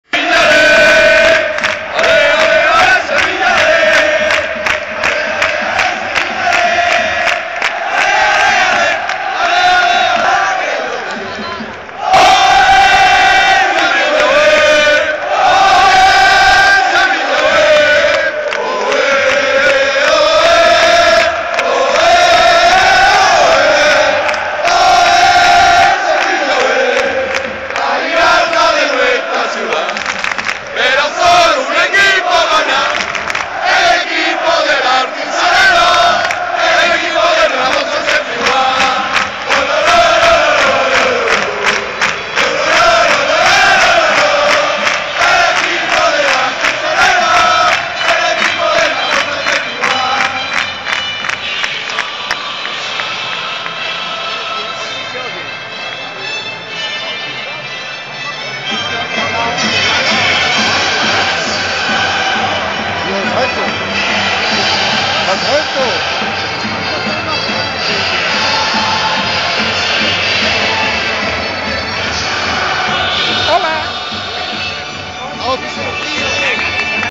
Video Nº 111
Prolegómenos en el Estadio Ramón Sánchez Pizjuán de Sevilla del partido de Champions League entre el Sevilla F.C. y el Arsenal que finalizó con el resultado de 3-1. El Sevilla alcanza el liderato que le situará como campeón de su grupo